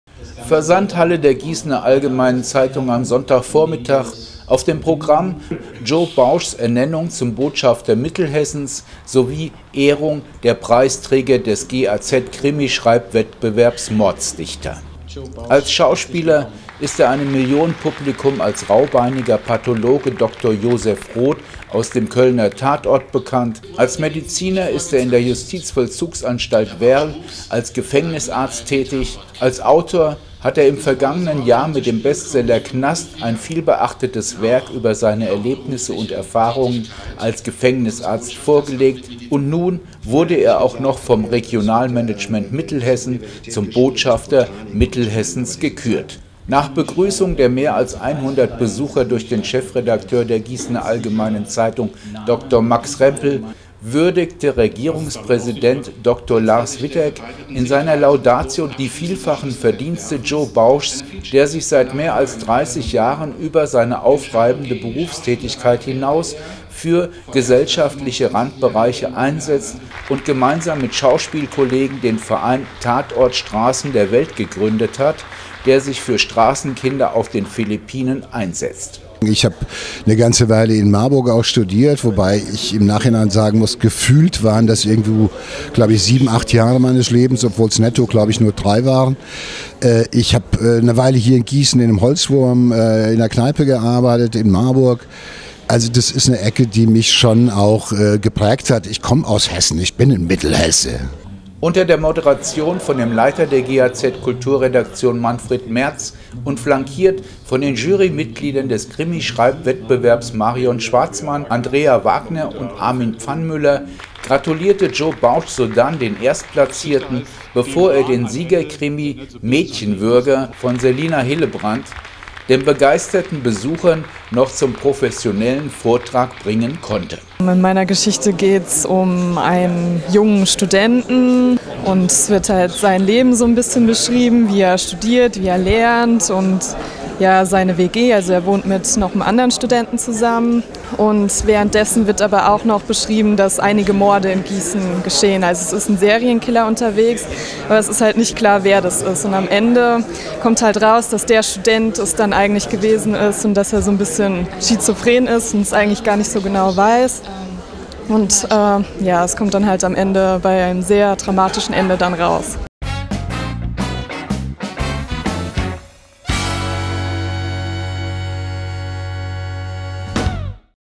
Joe Bausch bei der Gießener Allgemeinen
Gießen (cw). Im Rahmen der Preisverleihung des GAZ-Krimischreibwettbewerbs "Mordsdichter" wurde der als Pathologe des Köln-Tatorts, Autor und Gefängnisarzt in Werl bekannte Joe Bausch, zum Botschafter der Region Mittelhessen gekürt.